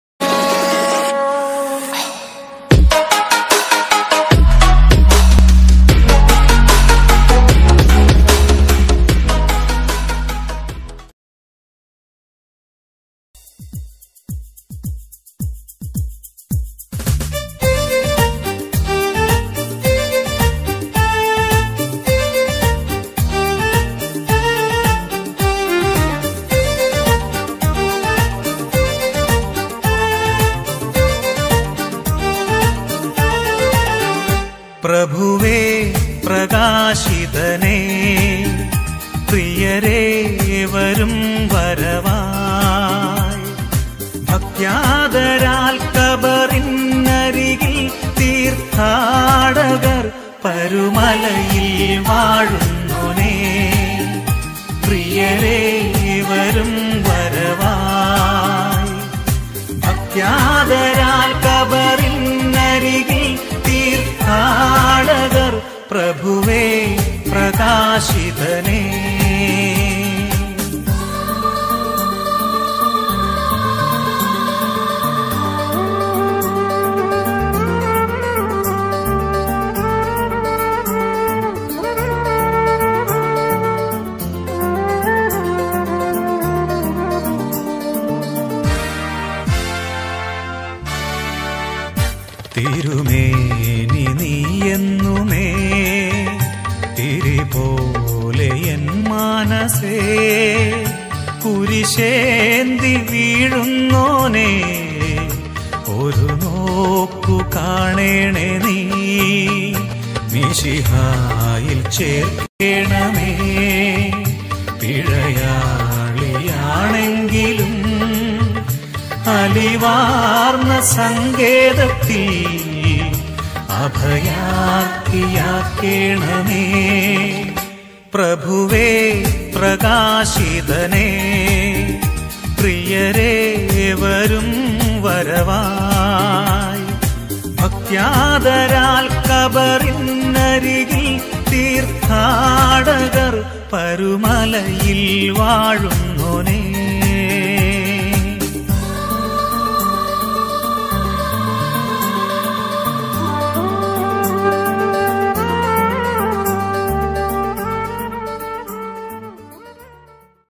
Pencil drawing of parumala thirumani
Saint Geevargese Mar Gregorios, popularly known as Parumala Thirumeni, was a Metropolitan of the Malankara Orthodox Syrian Church In 1947, Malankara Orthodox Syrian Church declared Mar Gregorios as a saint making him the first canonized
Born: 15 June 1848, Mulanthuruthy

Died: 2 November 1902, Parumala

Major shrine: St.Peter's and St.Paul's Orthodox Syrian Church, Parumala

Feast: 2 November

leaf cutting art of  village food channel firoz
chuttipara

m4tech jio joseph leaf cutting art